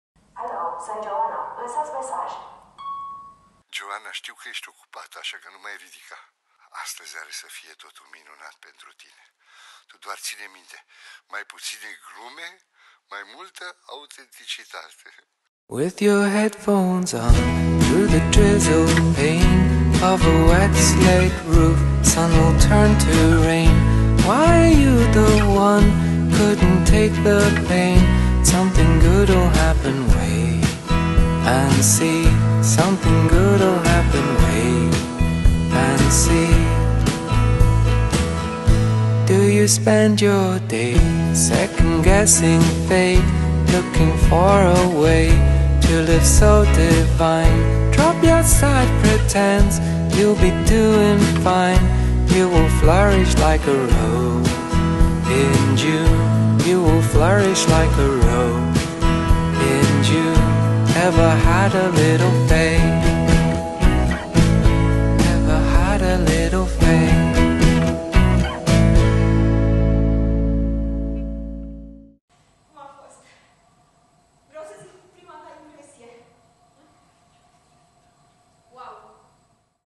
Trailer ''Treapta a noua'' Bulandra
„Treapta a noua” de Tom Ziegler, regia Anca Maria Colțeanu. Din distribuție fac parte actorii: Manuela Ciucur, Catinca Maria Nistor, Oana Ștefănescu, Adela Bengescu.

„Treapta a noua” e povestea unei femei care a ales  certitudinea incertului în locul realităţii de necontrolat, o poveste în care inexplicabilul lăsat în urma distanţării de acea realitate schilodeşte emoţionalul celor ce ne iubesc. E povestea unei relaţii speciale ce s-a pierdut şi pe care mamā şi fiicā încearcă, alternând între timiditate şi resentiment, s-o recupereze. Spectacolul propune o reinterpretare a temei vinovăţiei în familia contemporană.